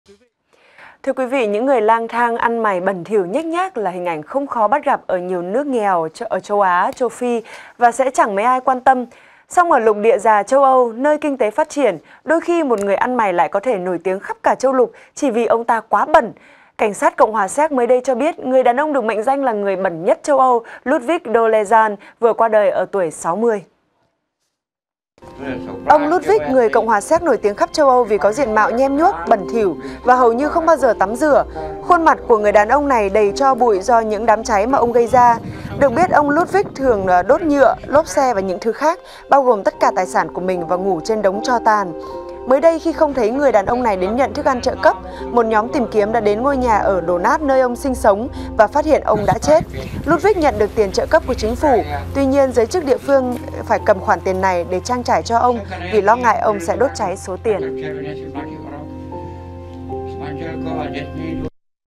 (VTC14)_Người đàn ông bẩn nhất châu Âu qua đời
(VTC14) - Ông Ludvik Dolezal, người CH Séc nổi tiếng khắp châu Âu vì có diện mạo nhem nhuốc, bẩn thỉu và hầu như không bao giờ tắm rửa, vừa qua đời ở tuổi 60. Được biết, ông Ludvik thường đốt nhựa, lốp xe và những thứ khác, bao gồm tất cả tài sản của mình và ngủ trên đống tro tàn.